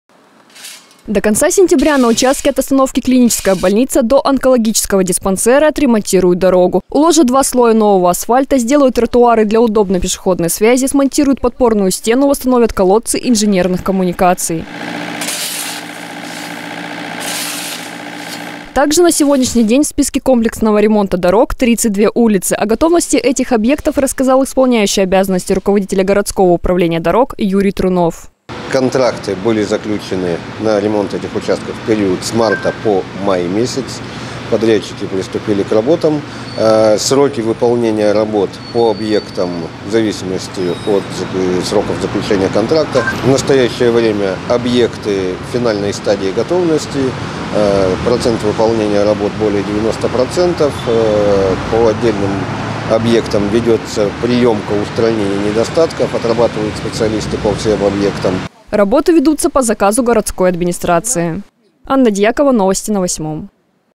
К онкодиспансеру во Владивостоке делают новую дорогу.
До конца сентября на участке от остановки «Клиническая больница» до онкологического диспансера отремонтируют дорогу, уложив два слоя нового асфальта, сделают тротуары для удобной пешеходной связи, смонтируют подпорную стену, восстановят колодцы инженерных коммуникаций.